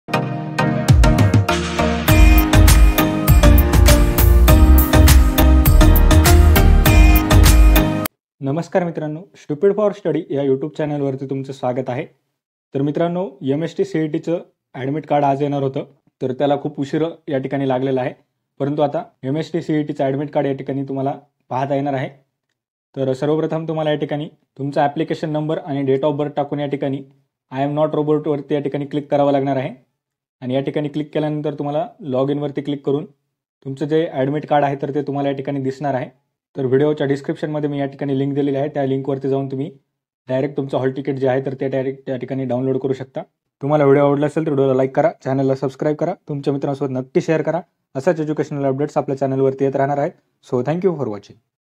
🔥MHTCET Admit Card Released | How To download? | Link to download | Big Update | MHTCET 2022-23
🔴Link To download Admit Card :-